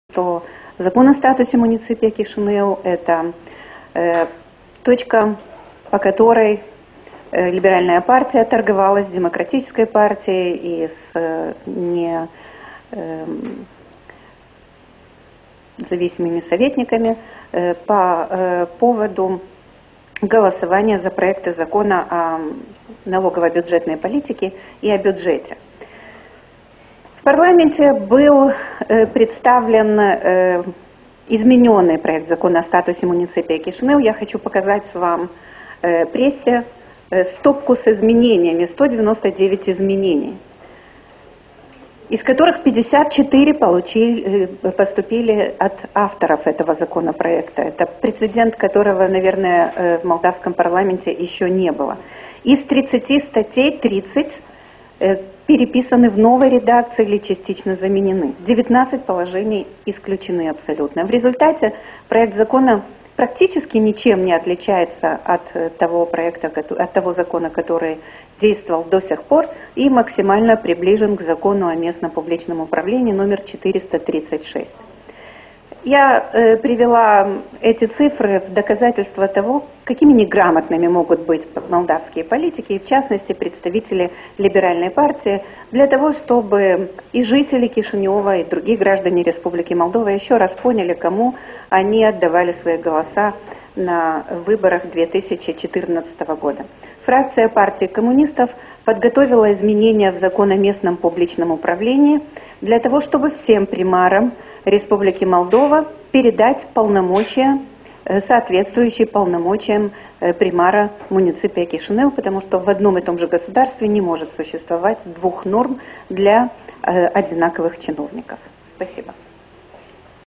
Declarațiile Elenei Bodnarenco în timpul ședinței Parlamentului Republicii Moldo...
Elena Bodnarenco: Votarea statutului municipiului Chișinău este un t&#226;rg politic &#238;ntre PL și PD
Titlul evenimentului: Declarațiile Elenei Bodnarenco &#238;n timpul ședinței Parlamentului Republicii Moldova din 17 iunie 2016